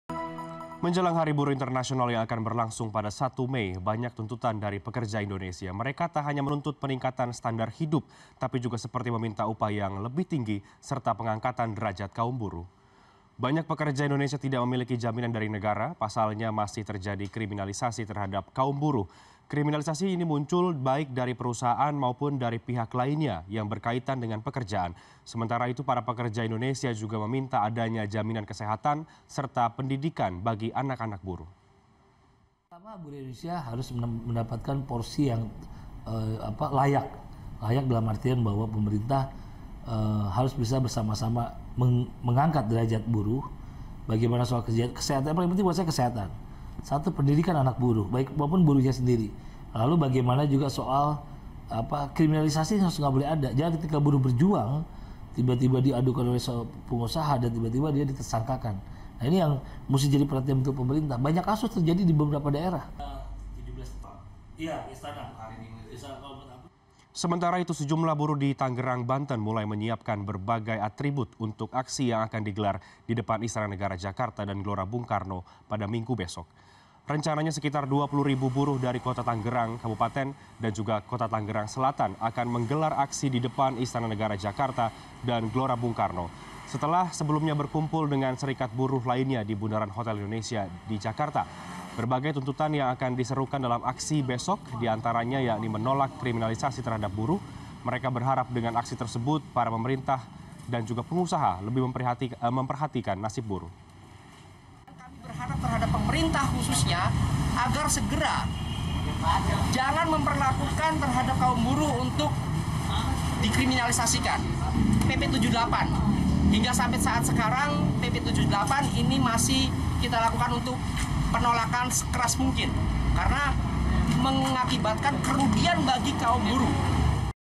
Jelang May Day dan Tuntutan Buruh
Menjelang hari buruh international yang akan berlangsung pada 1 Mei banyak tuntutan pekerja Indonesia. Mereka tak hanya menuntut peningkatan standar hidup, tapi juga seperti meminta upah yang lebih tinggi serta peningkatan derajat hidup.